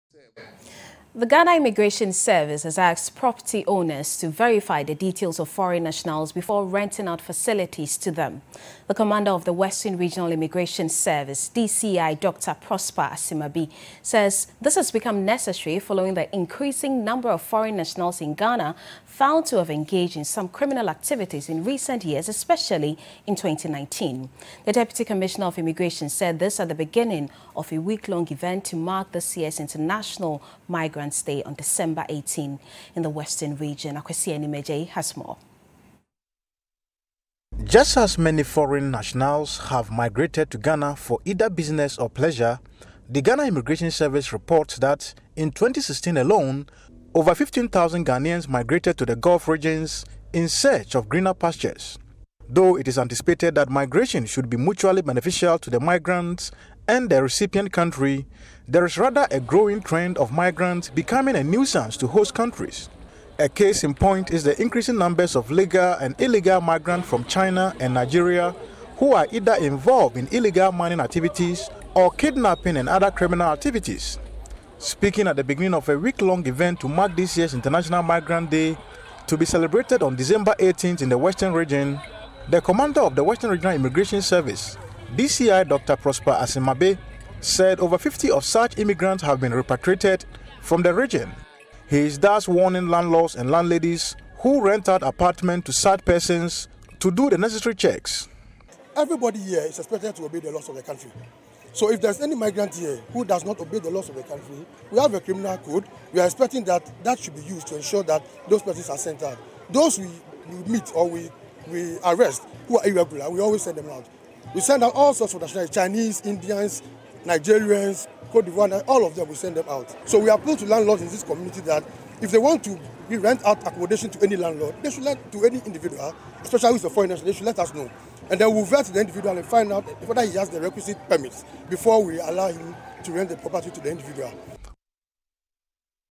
Immigration Service caution Landlords on renting to foreigners
As Sekondi-Takoradi hosts the 2019 International Migrants Day on December 18th, the Western Regional Immigration Service has kicked start it's sensitization against Irregular Migration with a float.